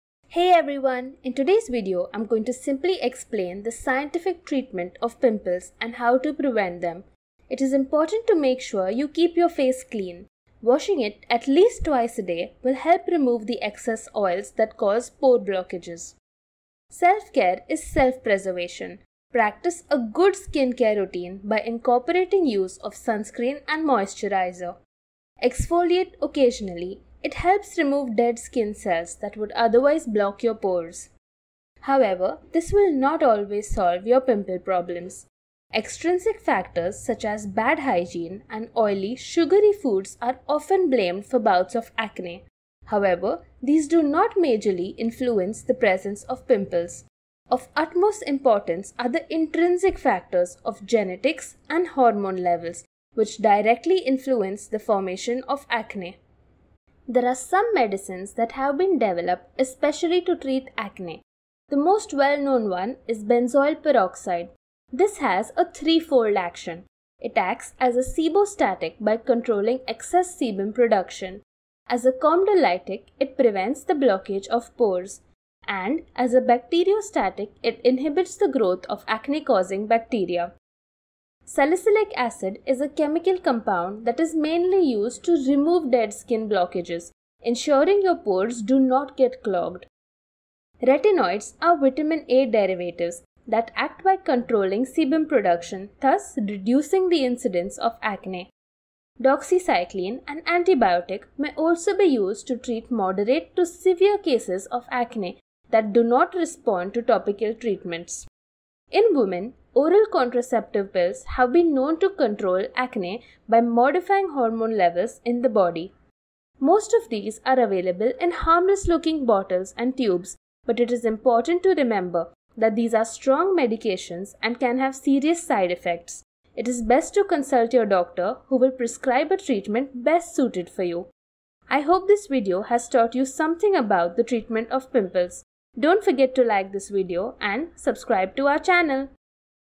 Pimples treatment / pimples removal on face at home / Treatment of pimples on face / Acne treatment
This video will answer a question all of you have been asking - how do we treat pimples? You’ll learn about simple measures to prevent them, what influence other factors have on this process, and scientifically proven medicine for acne - in just 2 MINUTES!

App: Available on android and iOS devices.